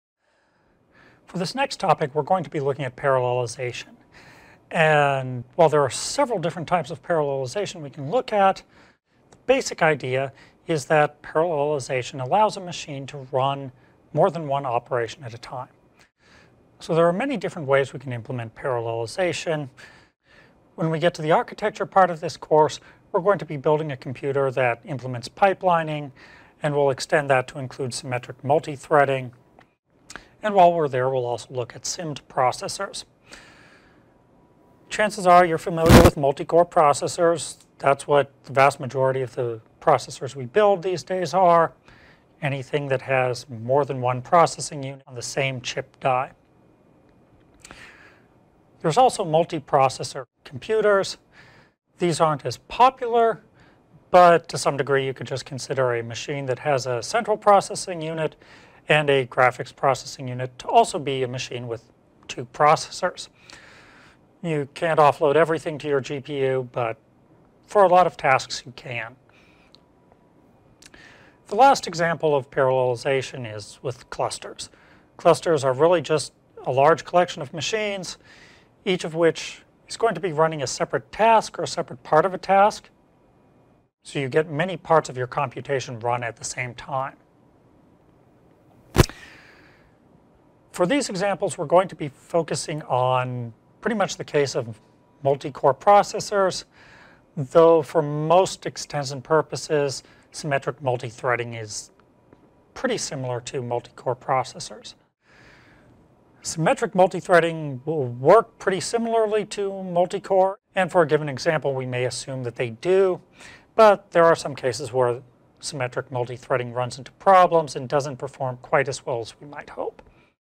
8. Parallelization
Conceptual discussion of how to calculate execution time when using parallel processing on a workload.